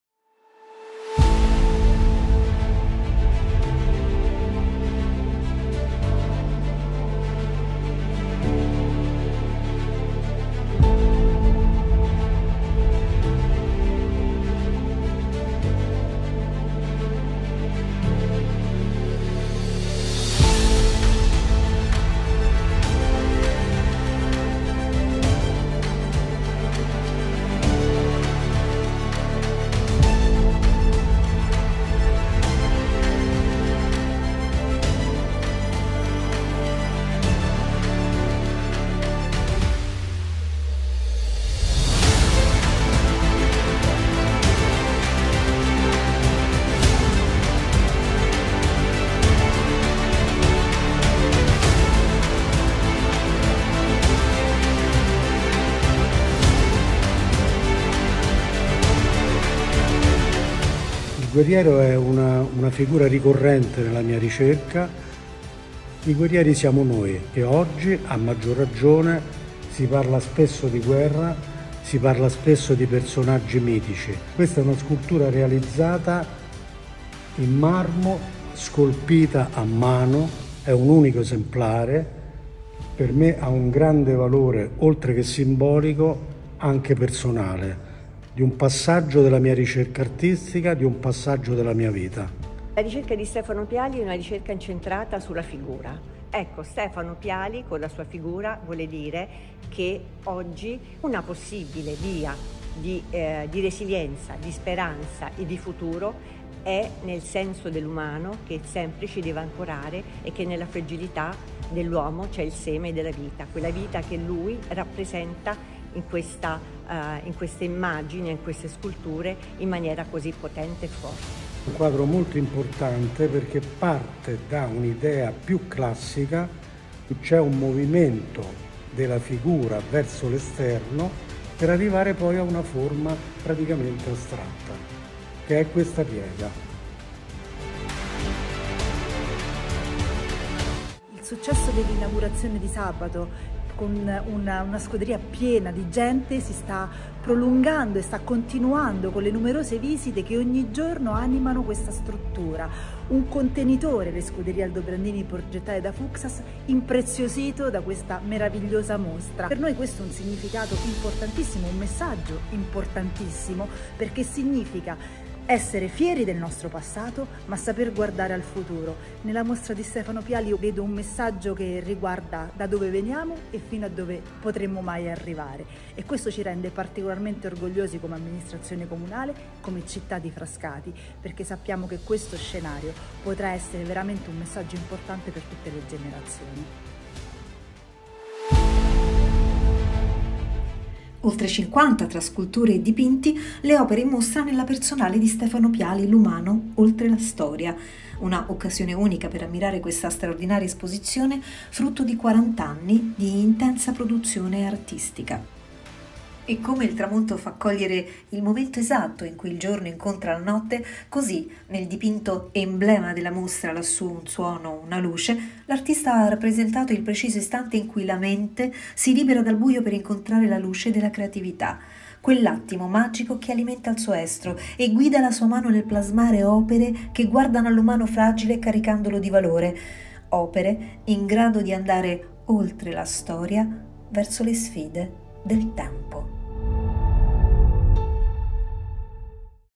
STEFANO PIALI - L’umano oltre la storia
Si è aperta sabato 03 febbraio 2024, con un grande successo mediatico e di pubblico, la personale di Stefano Piali “L’umano oltre la storia” visibile fino a domenica 25 febbraio nello spazio espositivo delle Scuderie Aldobrandini di Frascati.

In mostra oltre cinquanta, tra sculture e dipinti, dell'artista romano Stefano Piali, un’occasione unica per ammirare la sua straordinaria produzione artistica, frutto di quarant’anni di attività.

Lo spazio museale delle Scuderie Aldobrandini si è rivelato perfetto per apprezzare appieno i marmi bianchi di Carrara, le sculture in bronzo a cera persa o realizzate con terracotta patinata e ferro raffiguranti sovrani, guerrieri, scudi in resina e bronzo, opere molto suggestive, alcune delle quali dai diametri e altezze importanti posizionate in modo suggestivo proprio all’ingresso del museo. Un sodalizio perfetto, dunque tra territorio, cultura e bellezza che rende ‘L’umano oltre la storia’ un'esperienza da non perdere.

 “È con grande entusiasmo e onore che accolgo la straordinaria mostra dell’artista Stefano Piali  presso la nostra struttura museale di Frascati” ha dichiarato la Sindaca Francesca Sbardella che ha sottolineato inoltre: ” L’arte di Piali, come magistralmente delineato nel testo critico di Ida Mitrano, si distingue per la sua complessità e profondità con la capacità di coniugare le tecniche tradizionali, come la pittura e la scultura, con la contemporaneità delle sue visioni. Attraverso la sua opera ci invita a riflettere sulle sfide della nostra epoca, incanalando le ansie e le speranze dell’umano contemporaneo. La mostra, che offre uno sguardo approfondito sulla ricerca di Piali attraverso gli anni, si presenta come un'occasione unica per il pubblico di immergersi nella profondità della condizione umana, oltre la Storia convenzionale.  Stefano Piali non solo si confronta con la tradizione e i maestri del passato ma li abbraccia e li trasforma in un linguaggio personale e contemporaneo. Incoraggio tutti i cittadini di Frascati e i visitatori a esplorare questa straordinaria mostra, a lasciarsi trasportare dalla bellezza delle opere di Piali. Che questa esposizione sia un catalizzatore per riflessioni profonde e significative sulla condizione umana e sul ruolo dell’arte nel plasmare il nostro futuro”.
Scuderie Aldobrandini
Piazza Guglielmo Marconi, 6  00040 Frascati/ (RM)

Ingresso gratuito
Periodo esposizione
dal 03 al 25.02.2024

Orario ingresso
dal martedì al giovedì 15:00-18:00;
venerdì, sabato, domenica 10:00-19:00

Giorno di chiusura
lunedì